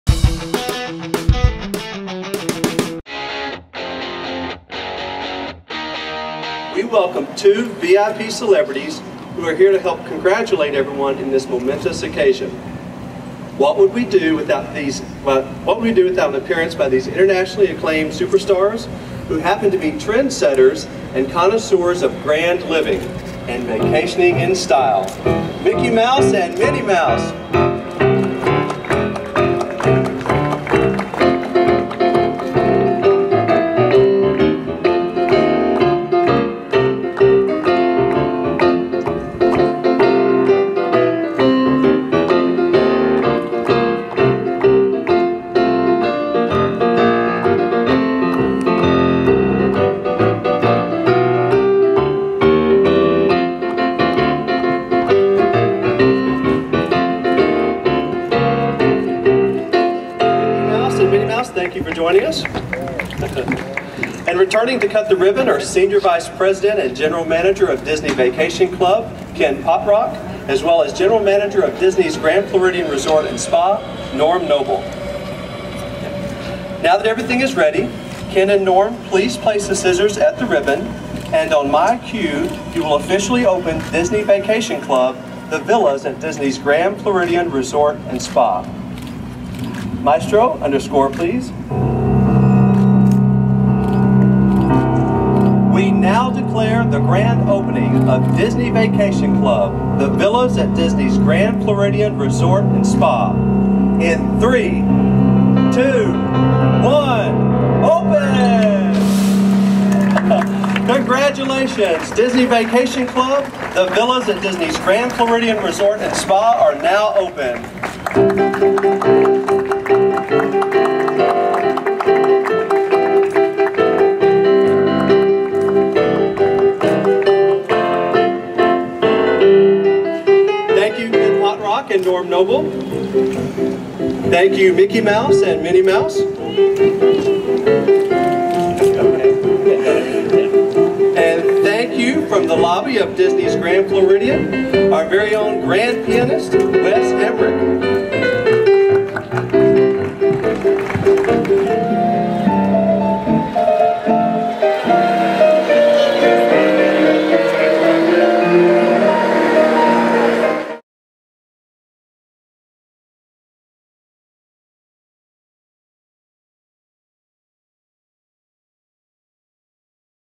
Mickey & Minnie cut the ribbon at The Villas at Disney's Grand Floridian Resort and Spa
Mickey Mouse and Minnie Mouse came to the grand opening of The Villas at Disney's Grand Floridian Resort and Spa. The Villas are part of the Disney Vacation Club, but rooms there can be rented by non-members too. This ribbon cutting was held on Oct. 23, 2013.